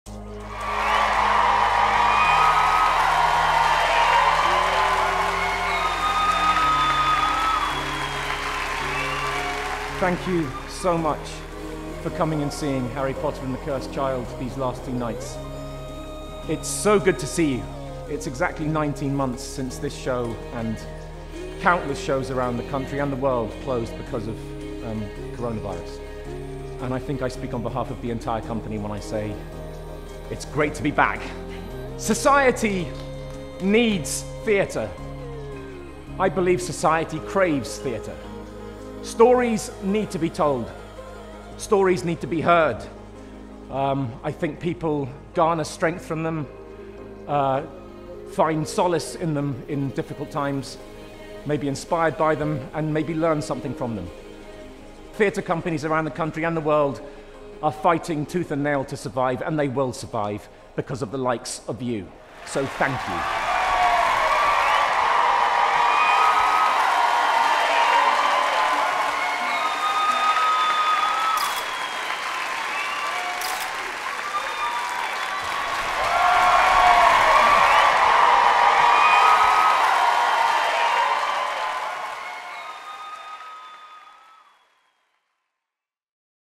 Cursed Child London’s First Curtain Call
CursedChildLDN took their first bow BackOnStage. Relive this magical moment with us once more!